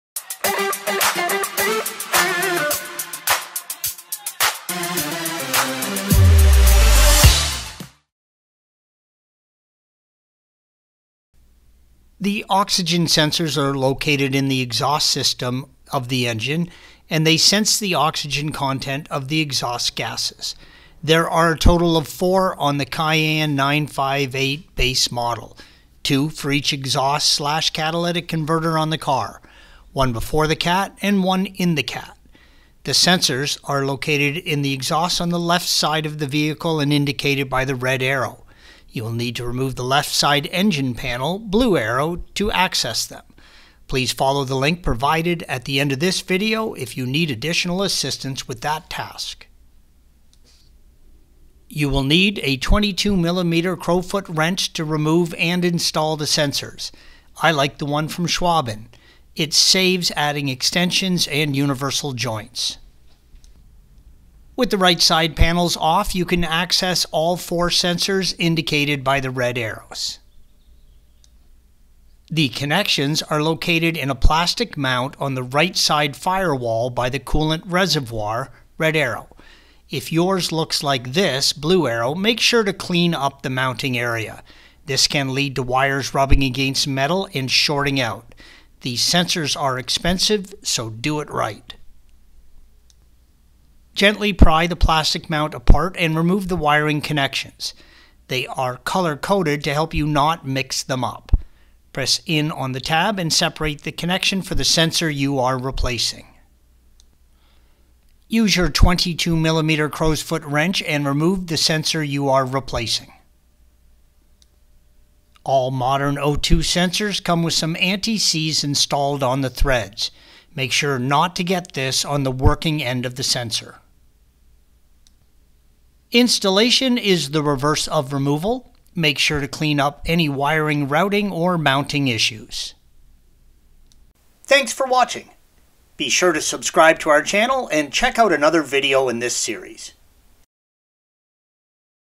Porsche 958 Cayenne VR6 Oxygen Sensor Replacement (2011 - 2018)
The oxygen sensors are located in the exhaust system of the engine, and they sense the oxygen content of the exhaust gases. There are a total of four on the Cayenne 958 base model. Two for each exhaust / catalytic converter on the car. One before the CAT and one in the CAT.

Project Time: 2hr
Tab: $0
Tools: 22mm Crowfoot wrench
Parts Required: Oxygen Sensors
Performance Gain: Proper Functioning Engine
Complementary Modification: Check your plugs